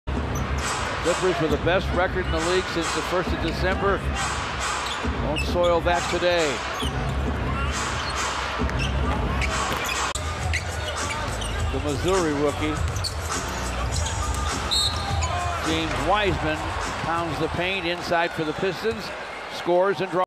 James Wiseman player Full Highlights vs CLIPPERS NBA Regular season game 02-02-2024
Check out the latest highlights of James Wiseman from the NBA preseason game between PISTONS vs CLIPPERS. Watch as James Wiseman was preforming on the court with his field goal attempts, rebounds, turnovers, steals, assists, and blocks. This video includes all of James  gameplays possessions from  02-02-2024.